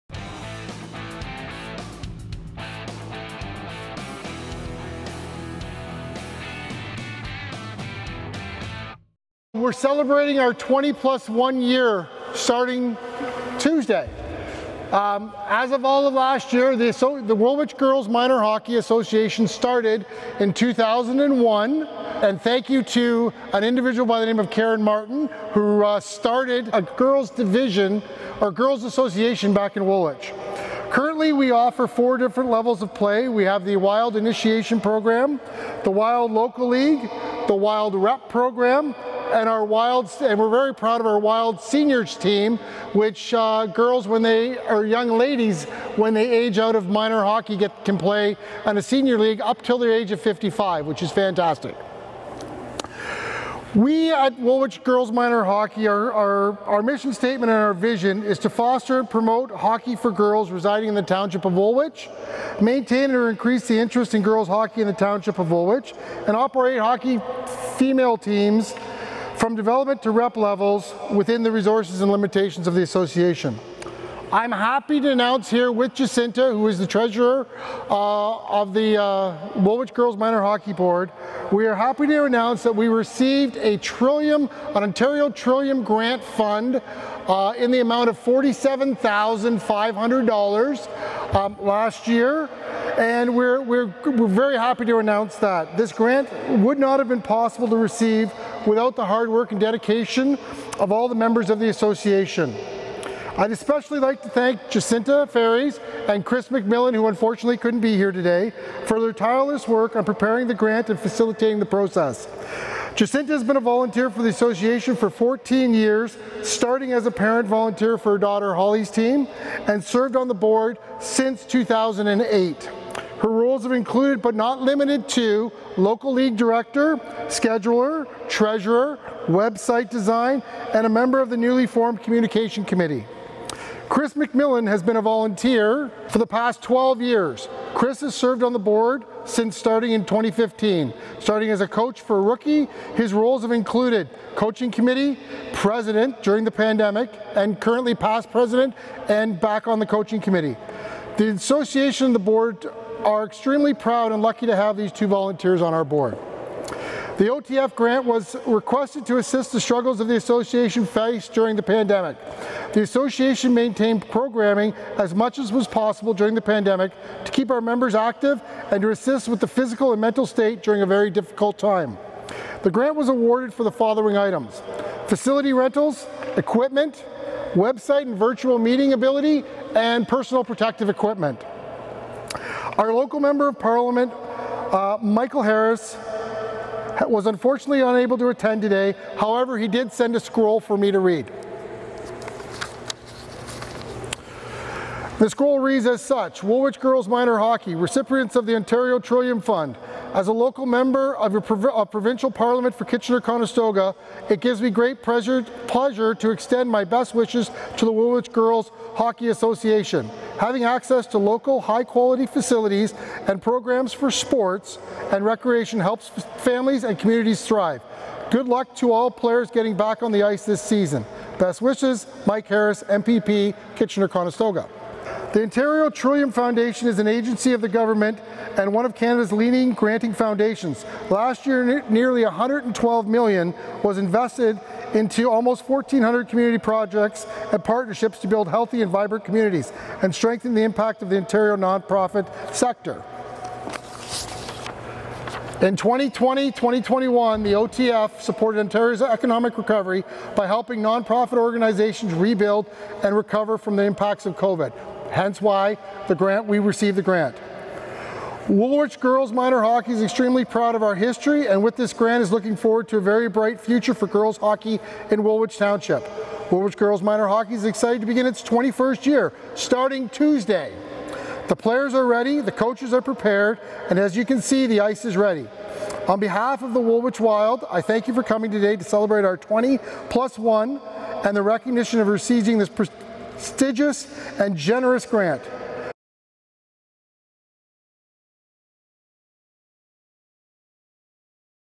Woolwich Girls Minor Hockey receives a grant from the Ontario Trillium Fund
Kevin Schmitt is the president of the Woolwich Girls Minor Hockey Association. He announced details about the OTF grant during. a special ceremony on Sunday September 5th at the Woolwich Memorial Centre.

The association is also celebrating a major milestone going into their 20th anniversary season.